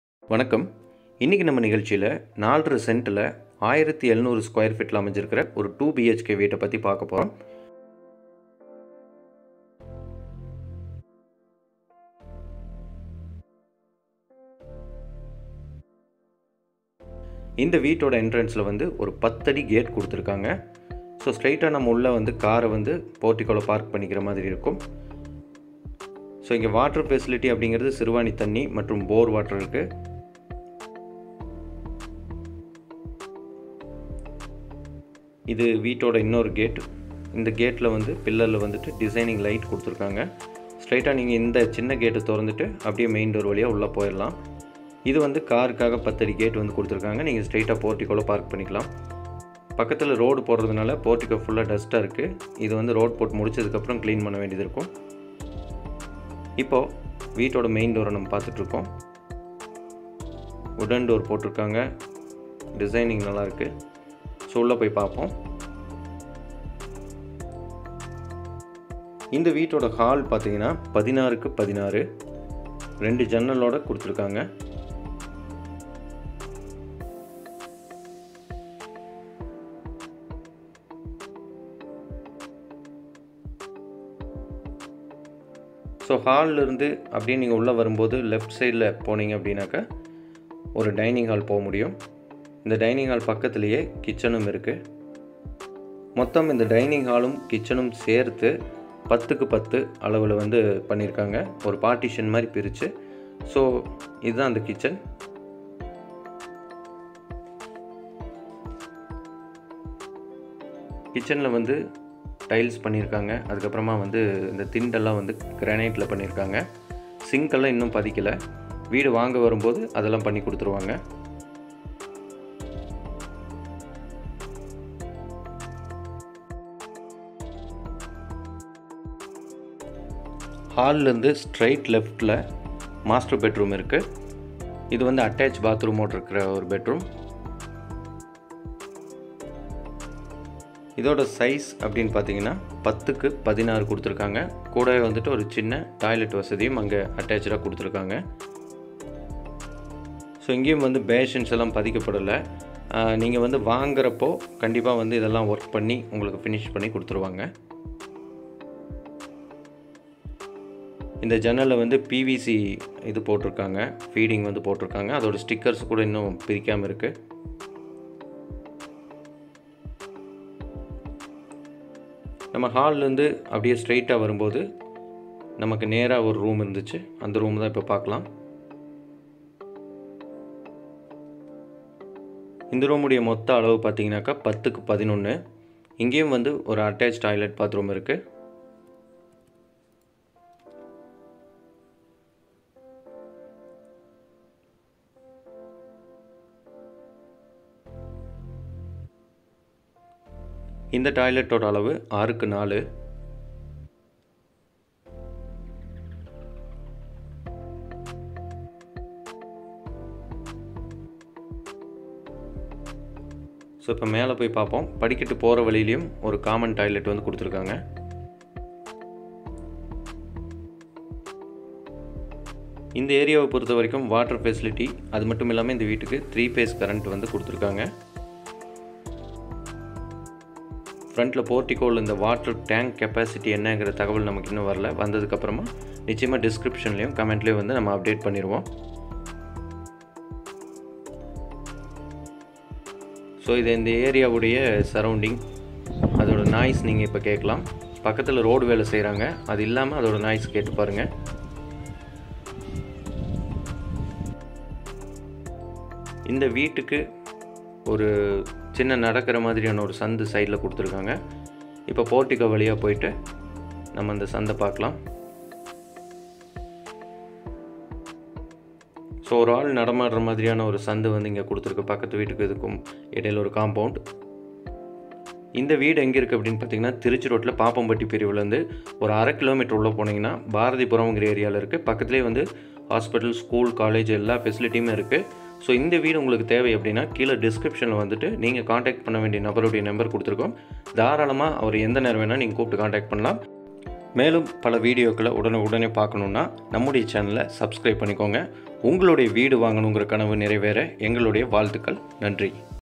House : 4.5 சென்டில் 1700Sqt 2Bhk வீடு விற்பனைக்கு
House Details :

Land : 4.5 Cents
Sqft : 1700
Portico : 16×10
Hall : 16×16
Kitchen : 10×10
Bedroom : 11×11
Master Bedroom : 10× 16
Bathroom : 6× 4
30 Feet Taar Road
Gated Community
Price 45Lakhs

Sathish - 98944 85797

Location Coimbatore

House Tour, House Design Tamil